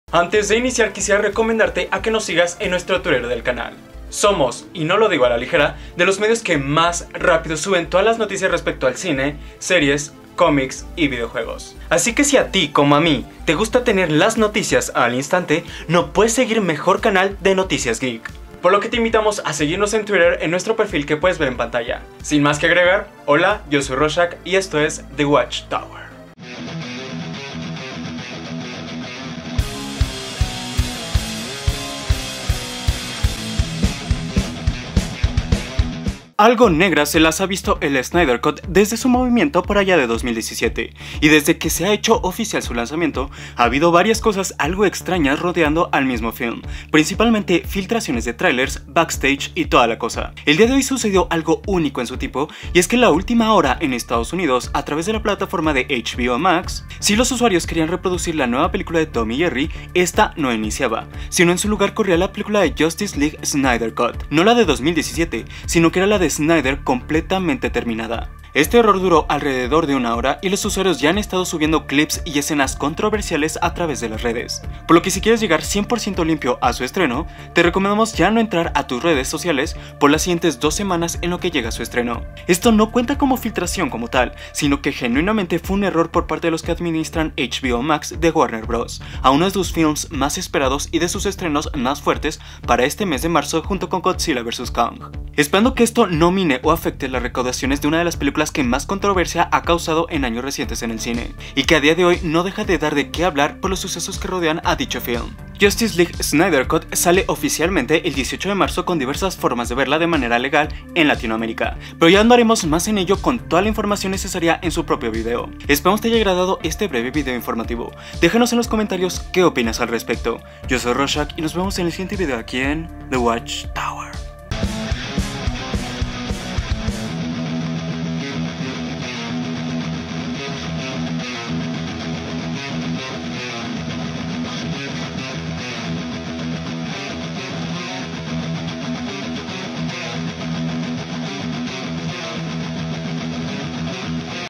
¡HBOMax sube POR ERROR el Snyder Cut!
Estamos en vísperas del estreno de SnyderCut en HBOMax y de diversas maneras a nivel internacional. Una de las películas más polémicas y esperadas por los fans de Dc desde la versión de Joss Whedon en 2017. Pero el día de hoy hubo un error en la plataforma liberando de manera completa y visuales terminados esta versión en lugar de Tom and Jerry. Por lo que en este breve video Análisis te traemos un resumen de esta noticia y cómo estuvo dicho acontecimiento.